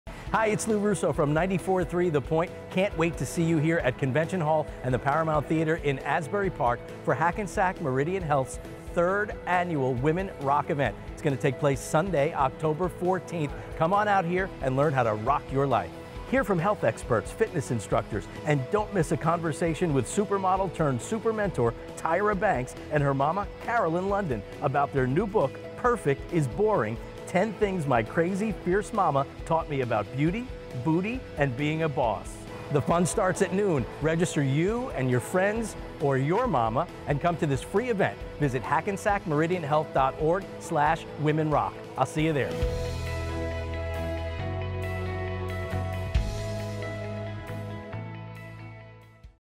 Lou Russo from 94.3 The Point Discusses Women ROCK
Join Lou Russo from 94.3 The Point at Hackensack Meridian Health's Women ROCK event on October 14 from noon-5 p.m. at Convention Hall and Paramount Theatre. Experience a women’s health and lifestyle event like you’ve never seen before. Visit HackensackMeridianHealth.org/WomenRock for more information and to register.